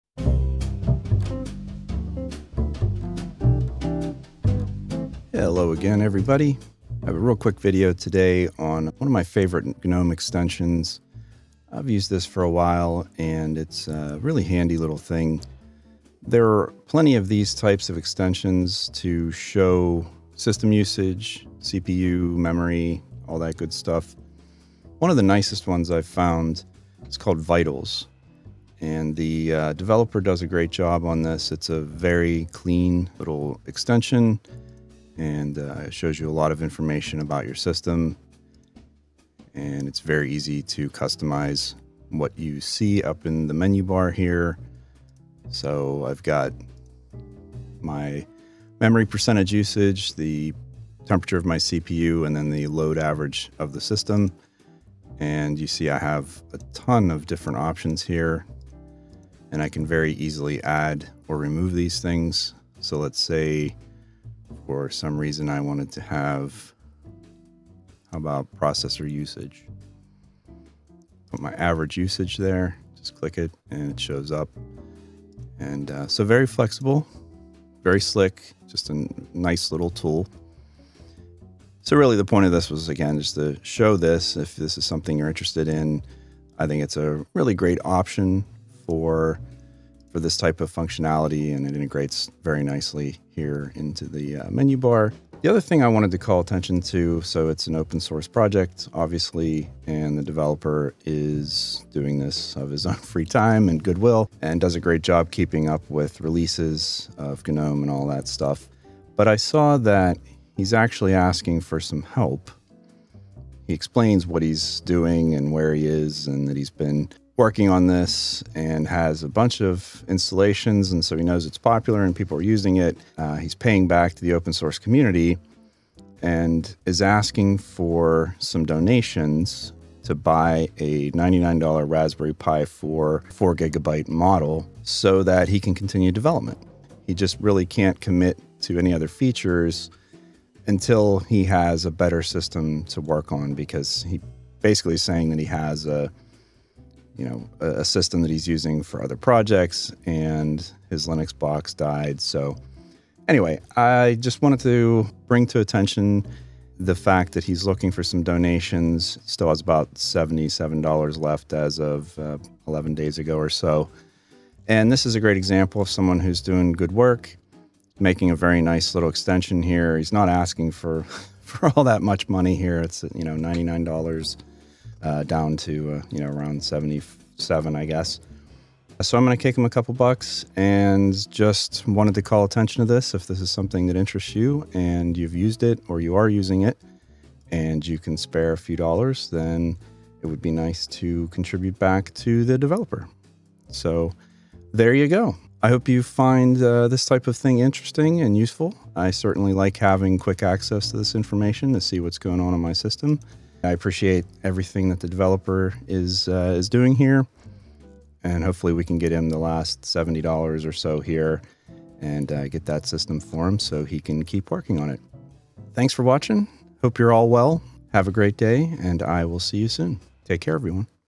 Fantastic GNOME System Monitor Tray - Vitals
One of my favorite GNOME Extensions is Vitals which is a system monitor that integrates into the top menu bar. It has a very nice UI, is easy to use and is well maintained by the developer. If you like having a quick and simple way to monitor your system this is an excellent option.

The developer is asking for donations to purchase a new system to develop this extension. He would like to buy a $99 Raspberry Pi 4 and is part way toward that goal. Let's see if we can get him all the way there!